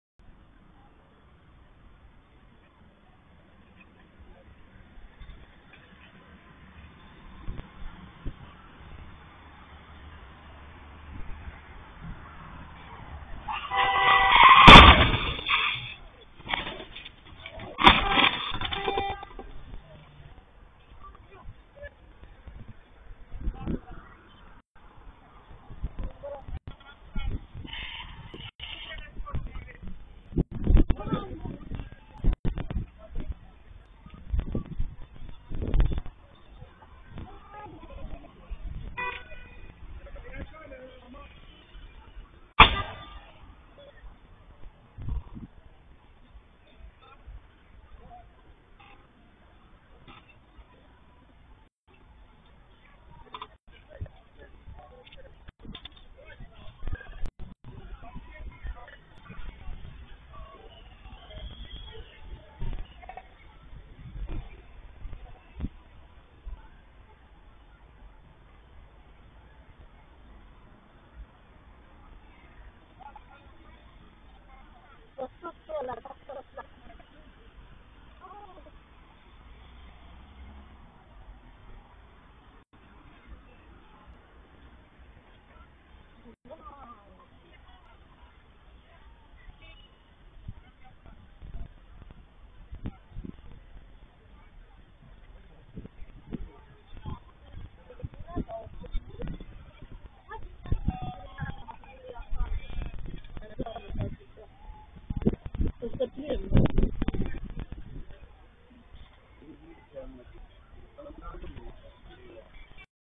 Accident pe strada Energiei din Bacau
Accidentul petrecut în urmă cu două zile, pe strada Energiei, din municipiul Bacău, s-a soldat cu doi răniți. Pe o cameră de supraveghere instalată de un locatar din zonă, a fost surprins momentul impactului.